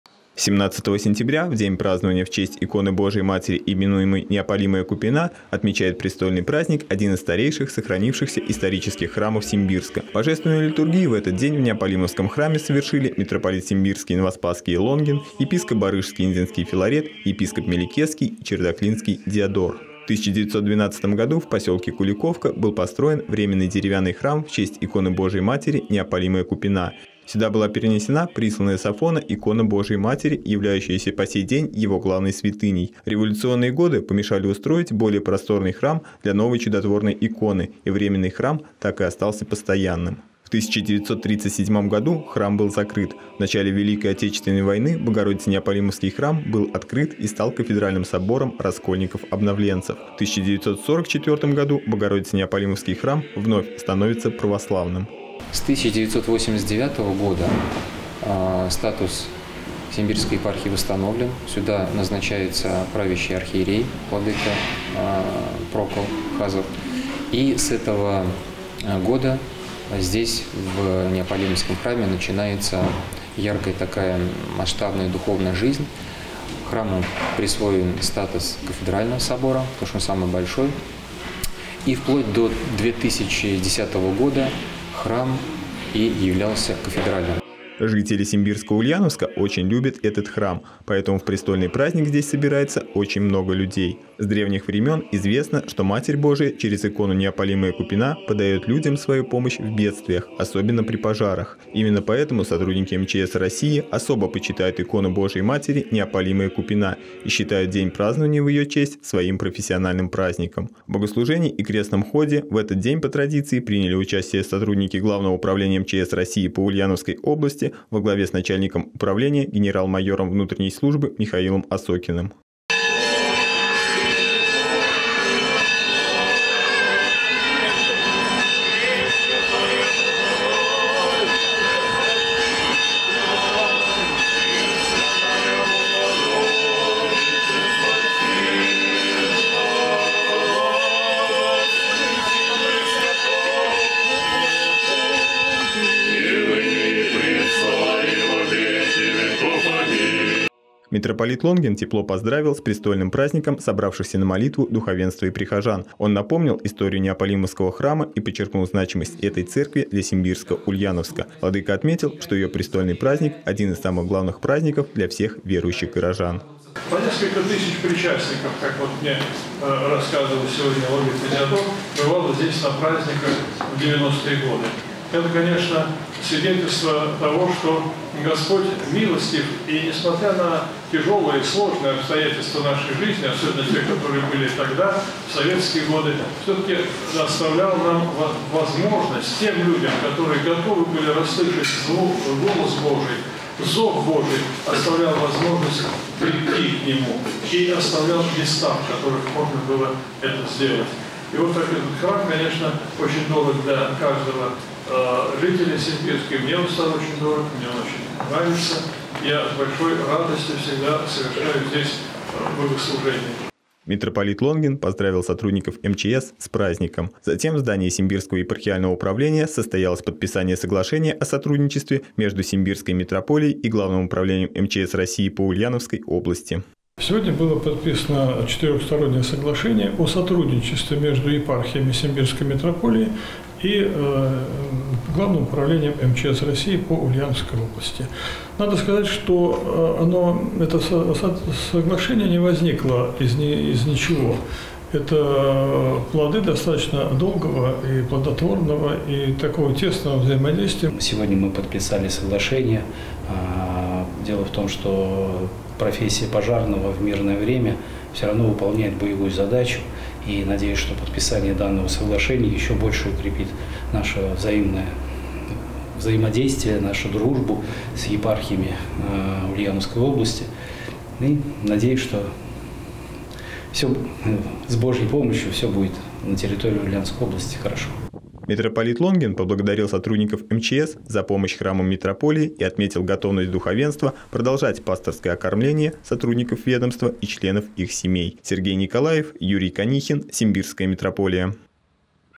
Старейший храм Симбирска-Ульяновска отметил престольный праздник
Храм в честь иконы Божией Матери «Неопалимая Купина» отметил престольный праздник. В этот день Божественную литургию в одном из старейших сохранившихся исторических храмов Симбирска совершили три Архипастыря Симбирской митрополии.